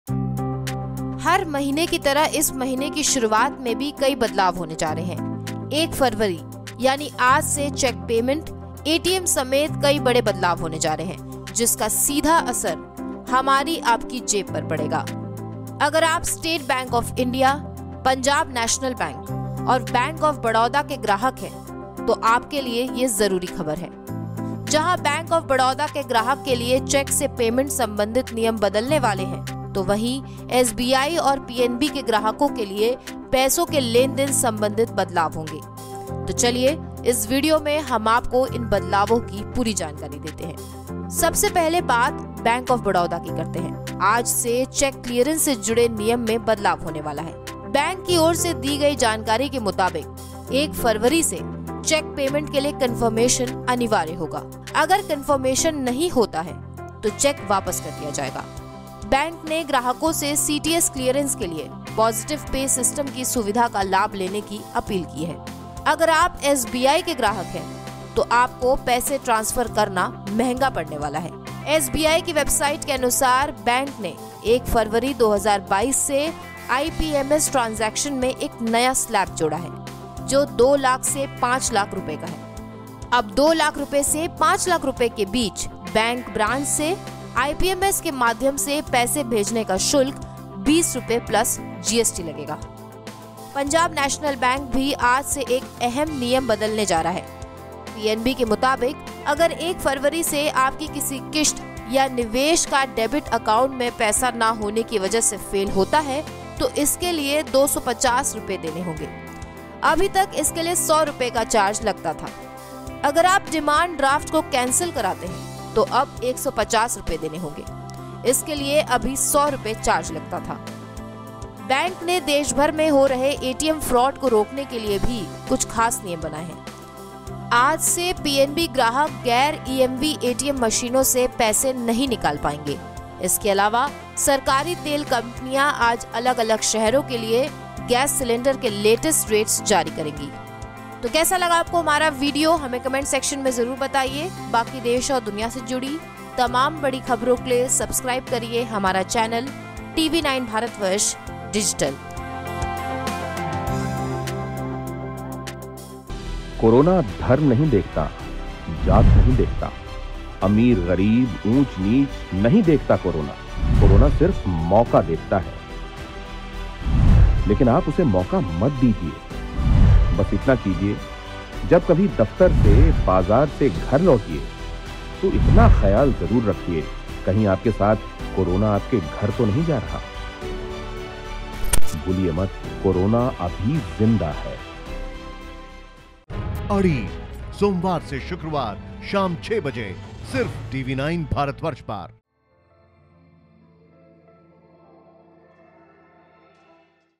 SBI, PNB, BoB के ग्राहकों के लिए जरुरी खबर! आज से बदल जाएंगे ये नियम
हर महीने की तरह इस महीने की शुरुआत में भी कई बदलाव होने जा रहे हैं. 1 फरवरी यानी आज से चेक पेमेंट, एटीएम समेत कई बड़े बदलाव होने जा रहे हैं, जिसका सीधा असर हमारी-आपकी जेब पर पड़ेगा.

Credits: National | AnushkaAgrawal/Producer | Ekta/Editor